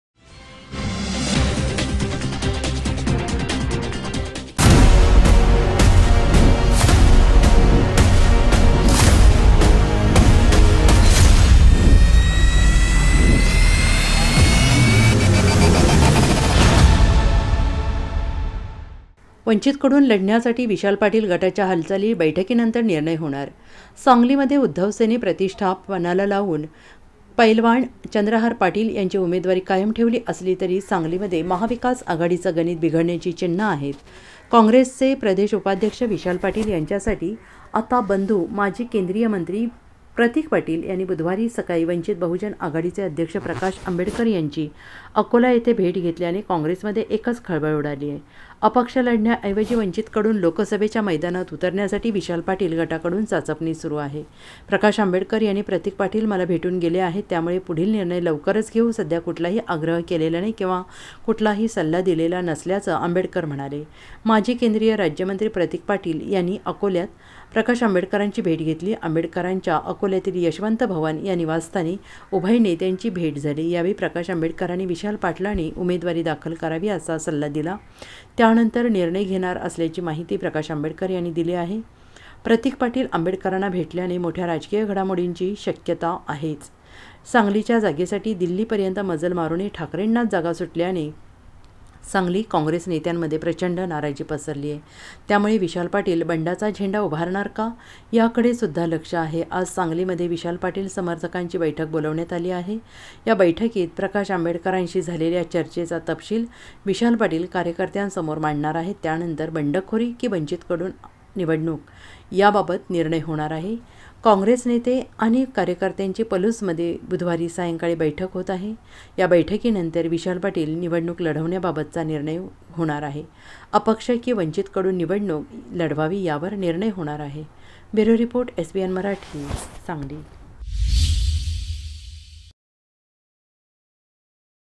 SANGLI | वंचितकडून लढण्यासाठी विशाल पाटील गटाच्या हालचाली, बैठकीनंतर निर्णय होणार
वंचितकडून लढण्यासाठी विशाल पाटील गटाच्या हालचाली, बैठकीनंतर निर्णय होणार